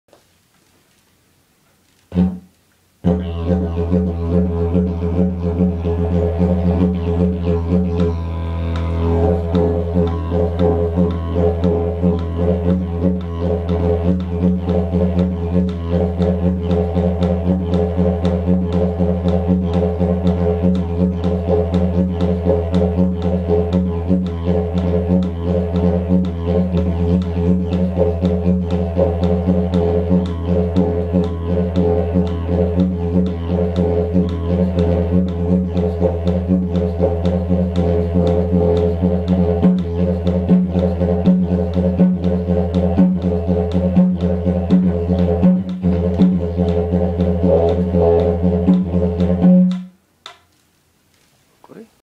Yidaki improvisation
Trying to make tighten lip after months of blank. My little cat Putty is listening (sleeping) next to me...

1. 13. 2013 in Tokyo